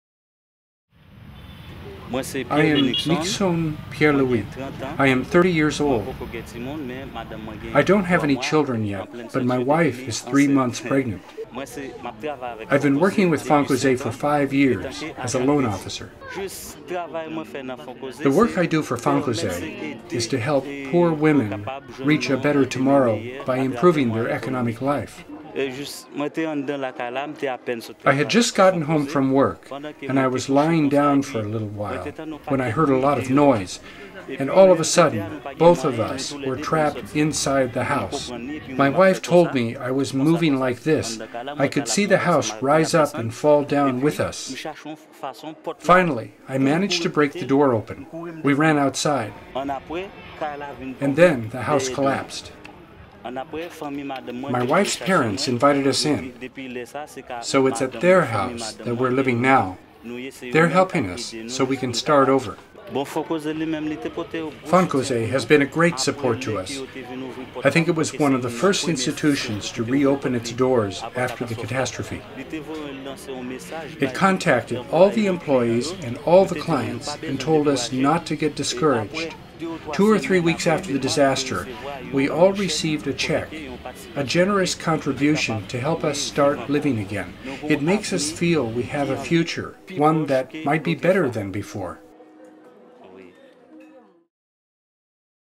Fonkoze: Nickson Pierre-Louis
One of fourteen post-earthquake sketches for Fonkoze, a dynamic microfinance institution working in Haiti.